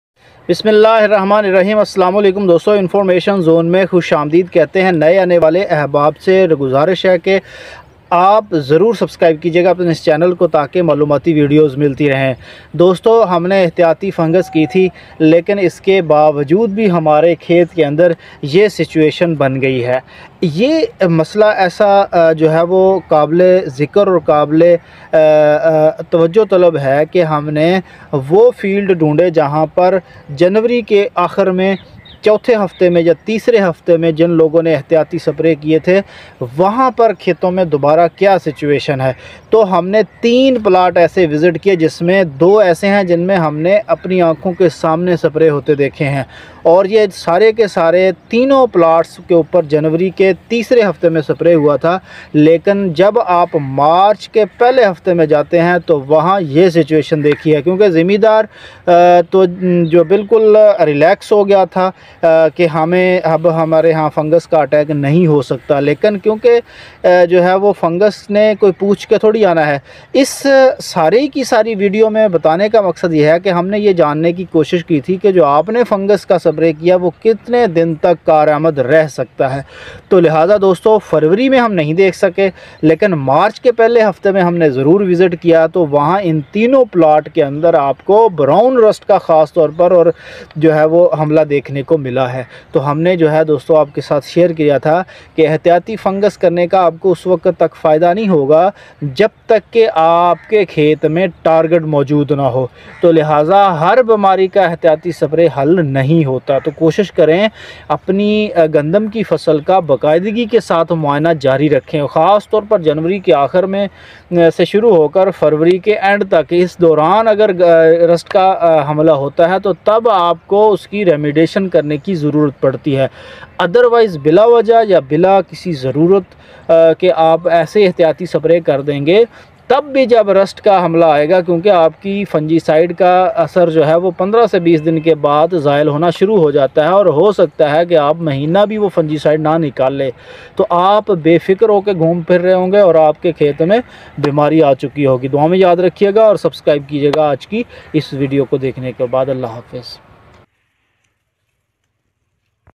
Wheat Spray Effect. Demo Plot Results. Rust response against Spray. Fungicide Effectiveness on Wheat
in this video we have discussed the results of 3 demo plots where we have seen the spray of fungicides on the plot in 4th week of the February. The results have been discussed after observation of these plots in the 1st week of March.

Fungicides used
1. Triademifon
2. Azoxystrobin+ Tubeconazol

محترم کسان بھائیوں اس ویڈیو میں ہم آپ کو تین عدد کھیتوں کا آنکھوں دیکھا حال بتائیں گے جس میں فروری کے آخری ہفتے میں فنجی سائیڈ سپرے کی گئی تھی۔انہیں پلاٹس کو مارچ کے پہلے ہفتے میں دوبارہ معائنہ کرکے رزلٹ چیک کیے گئے اور اس ویڈیو میں آپ کے ساتھ شئیر کئے جا رہے ہیں ہیں۔ کسان بھائیوں گندم پر احتیاطی سپرے سے گریز کریں اور جنوری کے آخر اور فروری کے پورے مہینے کے دوران اپنی فصل کا باقاعدہ سے معائنہ جاری رکھیں کسی جگہ پر حملہ نظر آنے کی صورت میں زرعی ماہرین کی مشاورت سے مینجمنٹ کریں۔
 مزید ویڈیوز کے لیے اپنے یوٹیوب چینل کو ضرور سبسکرائب کریں اللہ حافظ